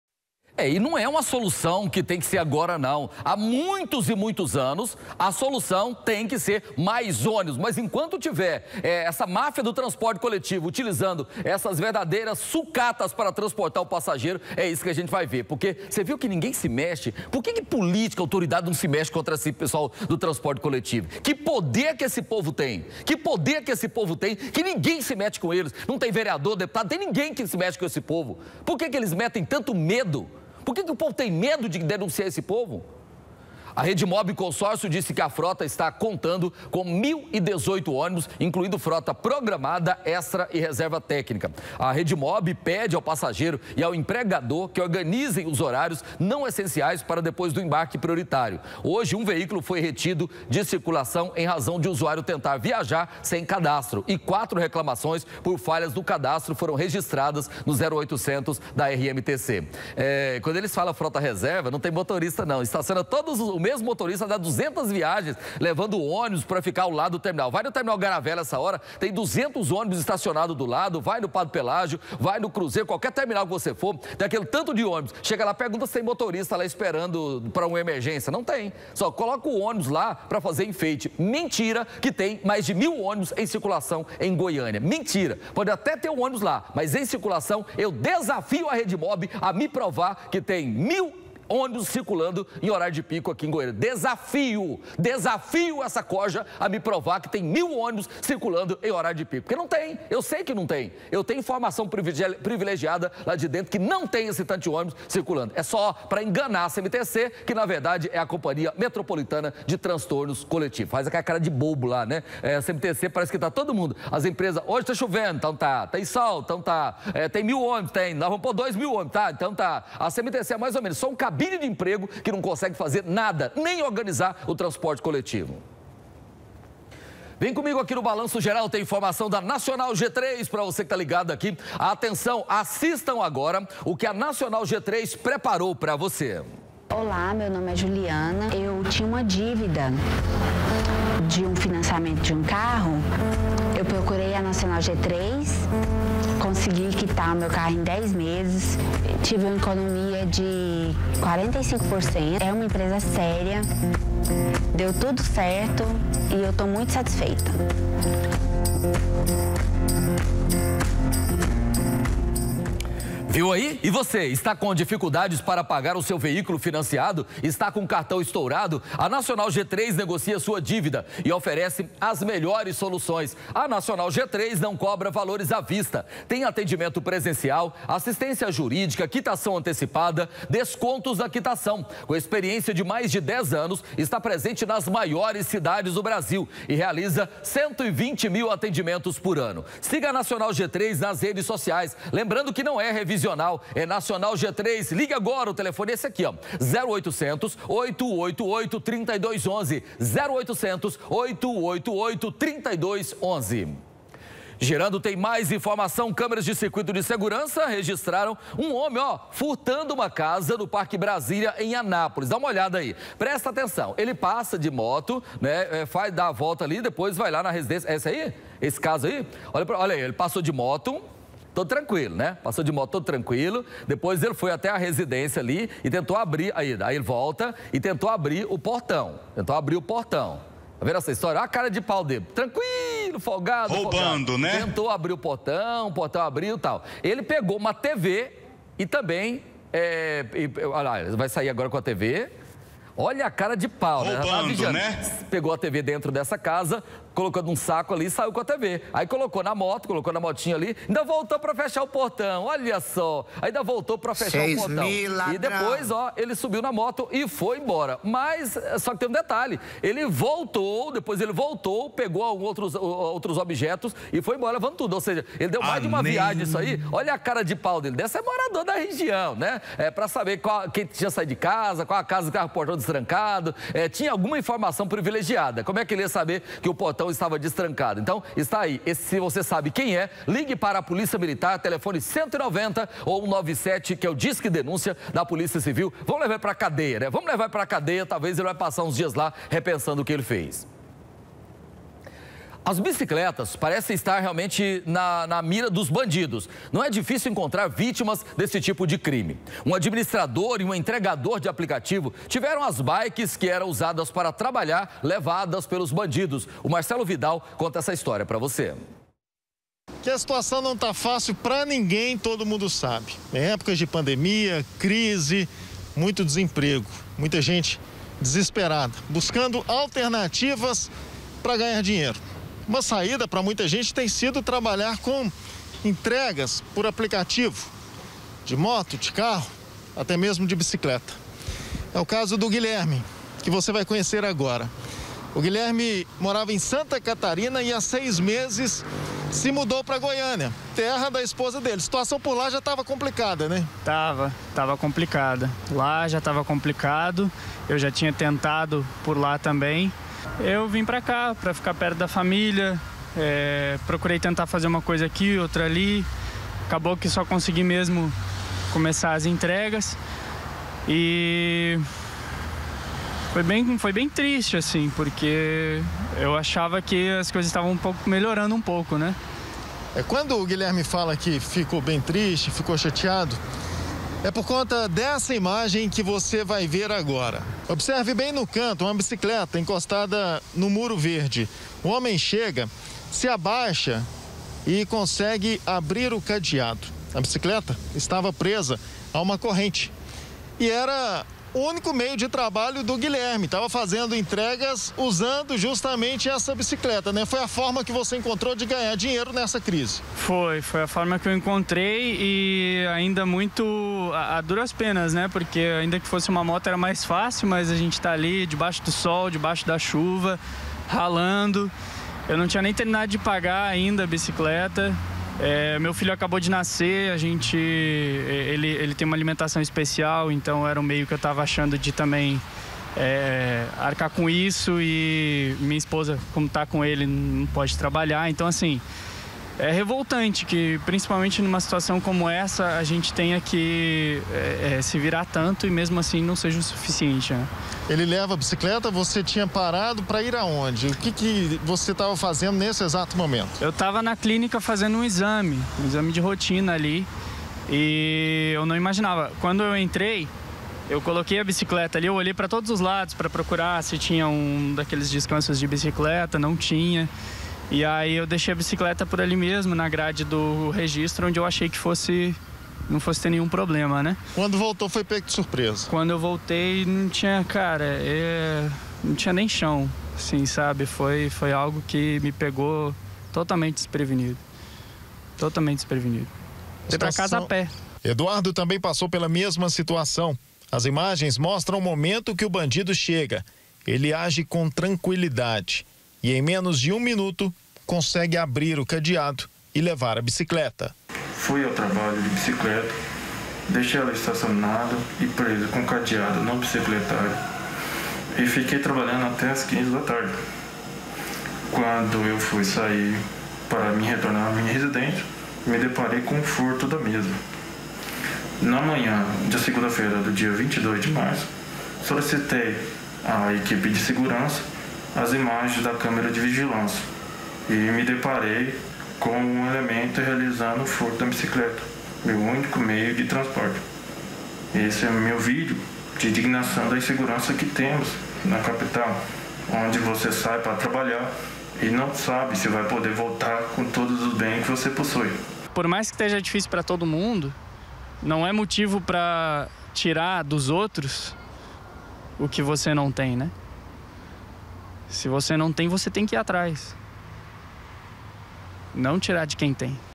FURTOS À LUZ DO DIA: BICICLETAS NA MIRA DE LADRÕES
AS BICICLETAS PARECEM ESTAR NA MIRA DOS BANDIDOS. NÃO É DIFÍCIL ENCONTRAR VÍTIMAS DESTE TIPO DE AÇÃO. UM ADMINISTRADOR E UM ENTREGADOR DE APLICATIVO TIVERAM AS BICICLETAS QUE ELES USAVAM PRA TRABALHAR ROUBADAS.